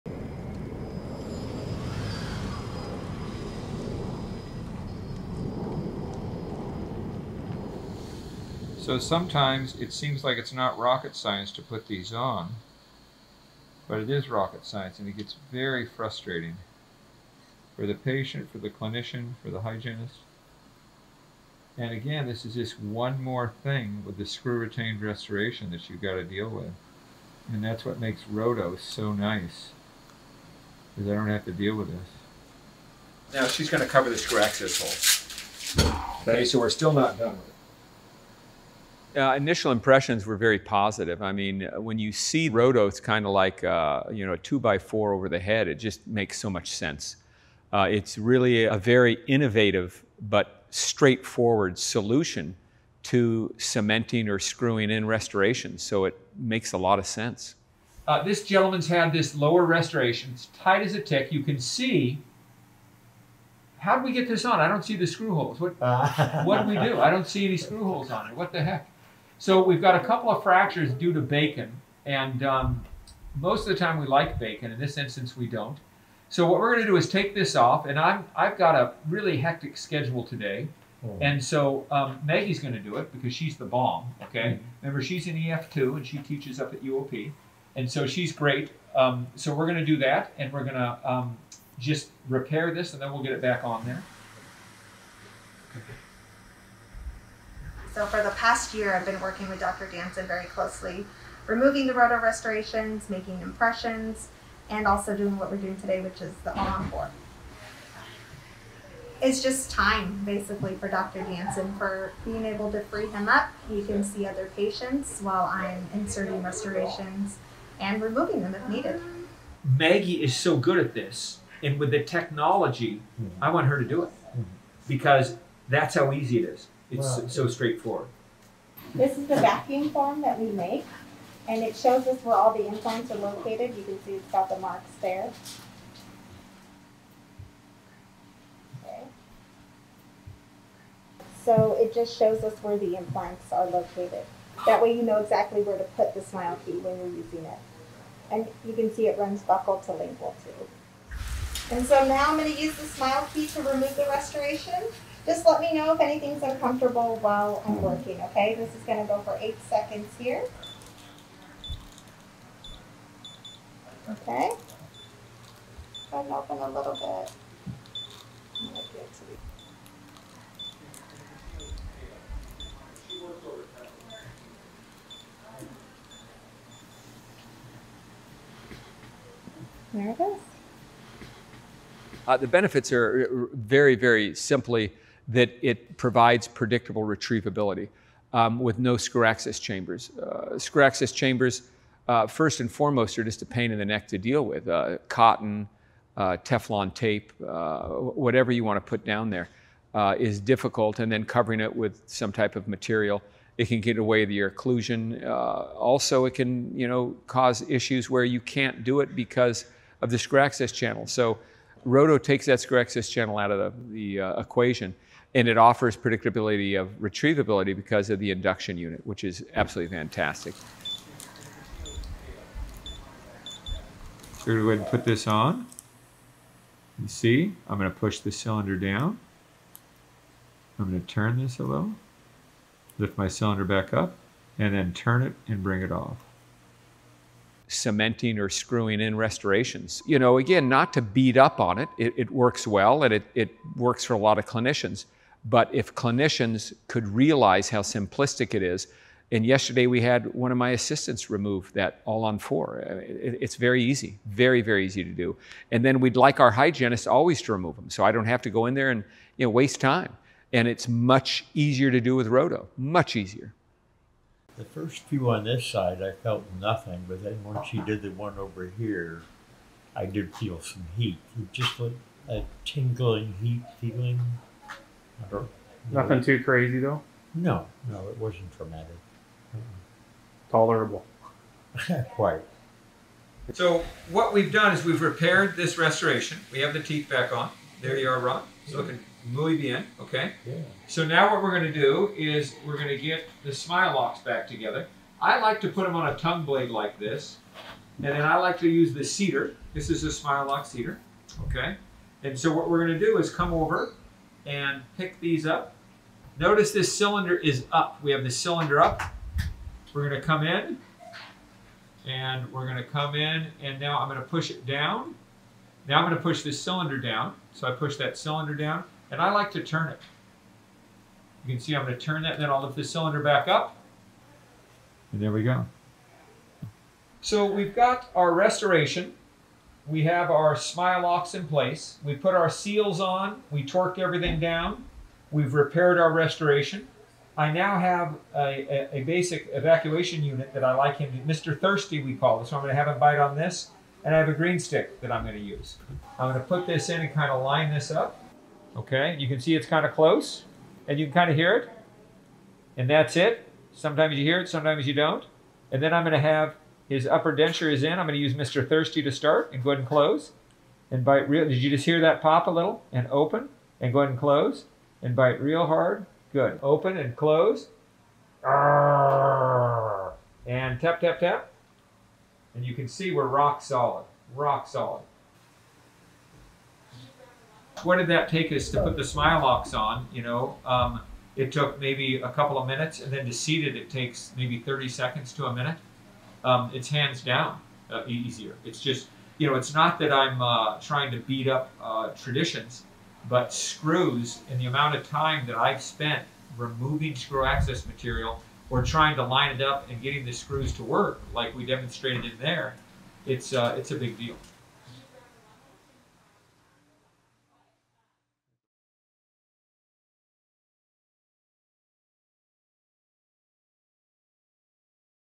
Dr. Curtis Jansen - The basics and benefits of RODO and the Smileloc® abutment system.
Dr. Curtis Jansen, CEJ Dentistry, takes you through the basics and benefits of RODO and the Smileloc® abutment system.

Smileloc® by RODO the no screws, no cement lightning fast retrieval abutment system available through The Aurum Group and RODO Digital Prosthetics Inc.

With the all-new Smileloc® by RODO, the world of implant dentistry has reached a new height. Utilizing a modern workflow, Smileloc® helps both patients and Dentists by delivering the highest quality of treatment outcome with significantly reduced chair time and complications.